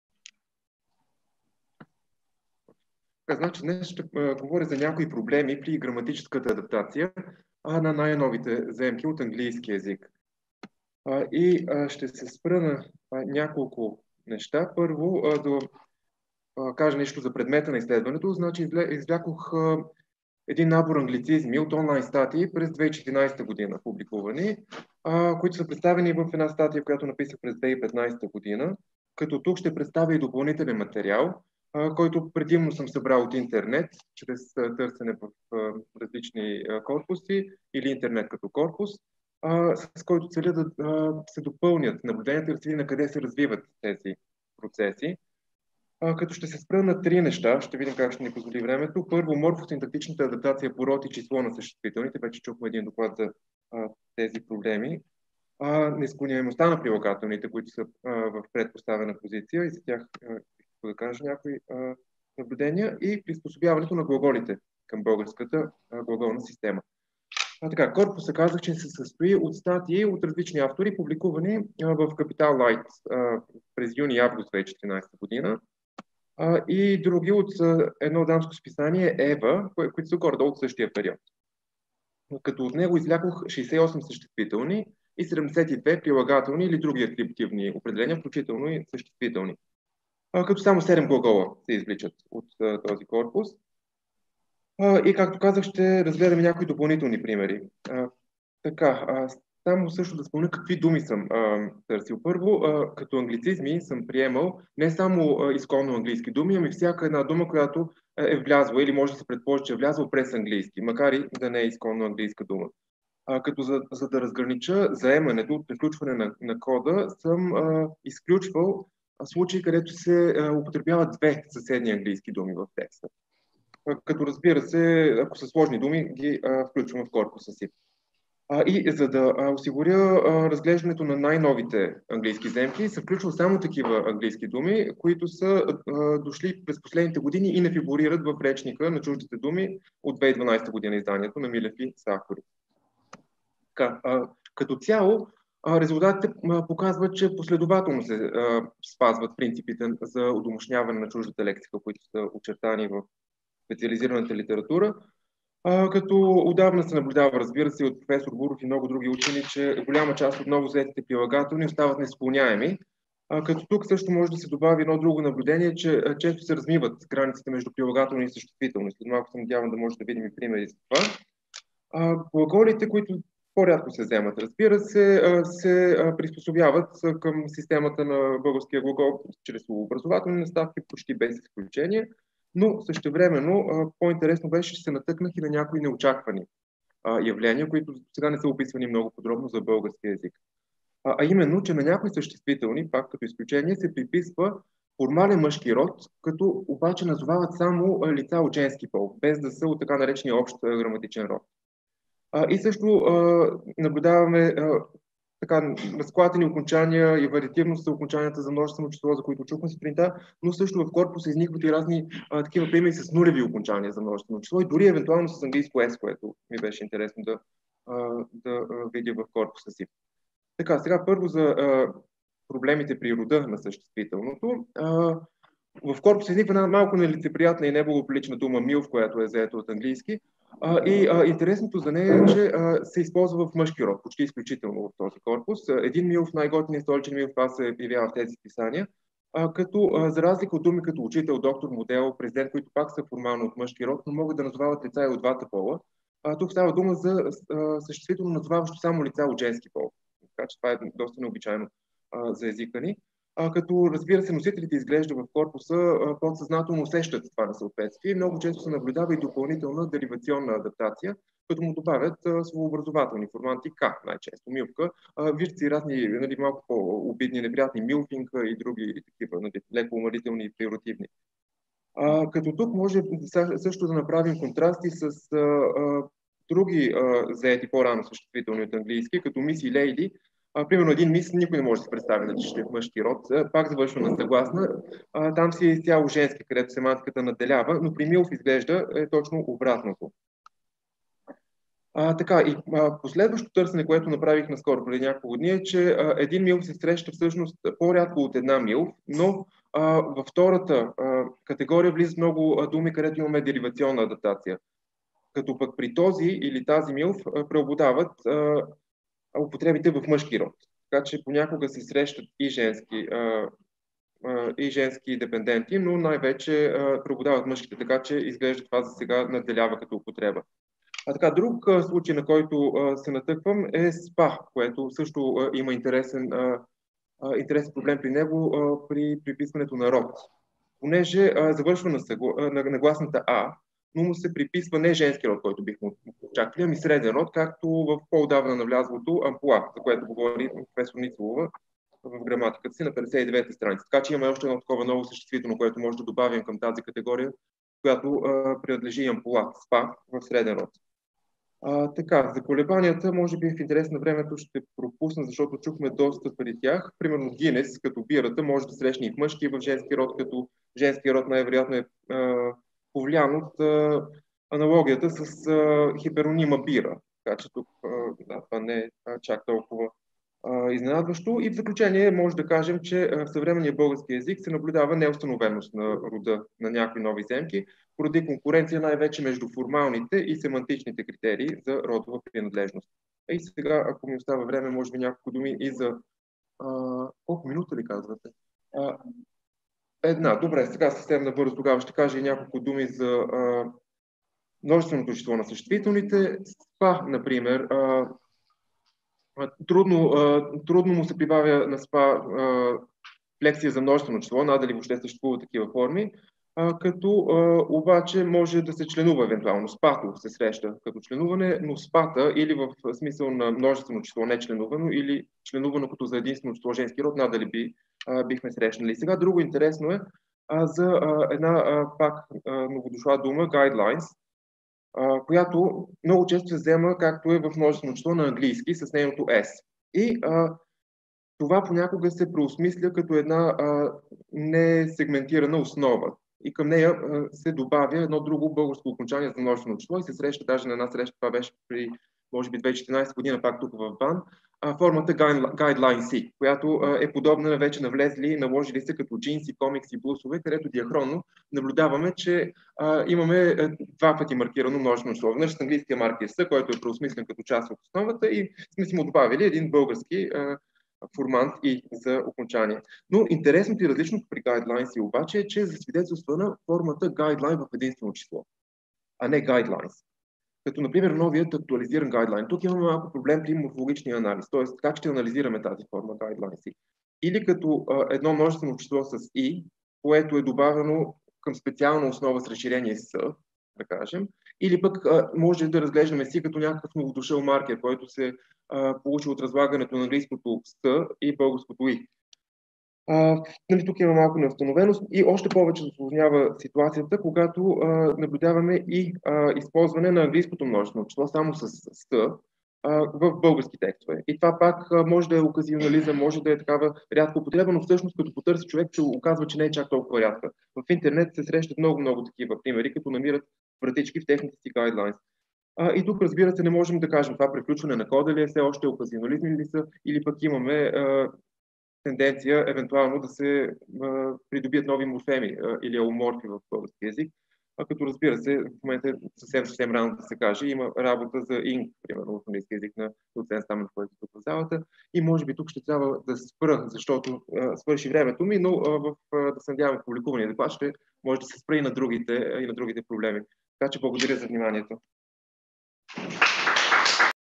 Гл. ас. Божил Христов (СУ): Морфосинтактична адаптация на най-новите заемки от английски в...
Гл. ас. Божил Христов (СУ): Морфосинтактична адаптация на най-новите заемки от английски в българския език